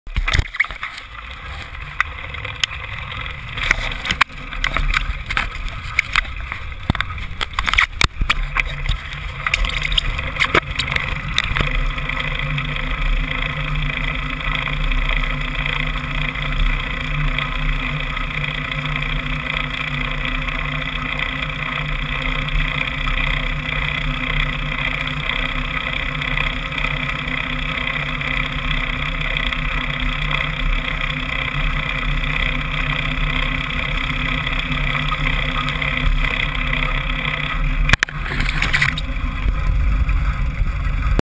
Route immergée
Route immergée entre Le Boulou et Arles sur Tech, lors du défi 2016